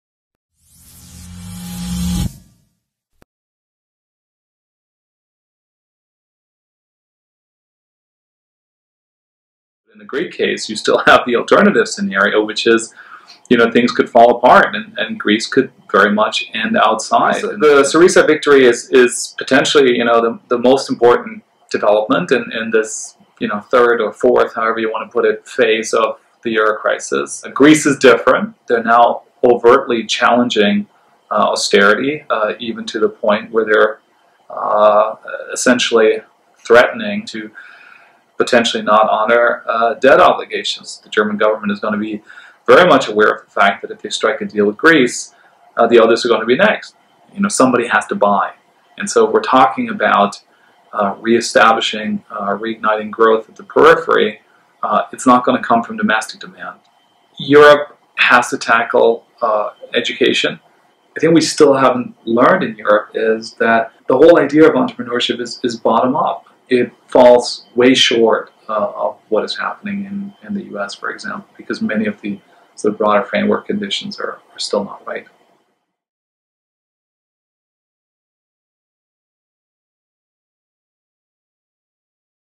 ESC@Yale: David Bach on Europe's Economic Future - TRAILER
David Bach, Senior Associate Dean at the Yale School of Management, discusses with us why Syriza's electoral victory is a crucial mark in the "history of the crisis," why there is no "one seize fits all" solution for Europe, and where we have to look in order to stimulate innovation and entrepreneurship.

Find out more about the European Student Conference, which will take place at Yale SOM from Feb.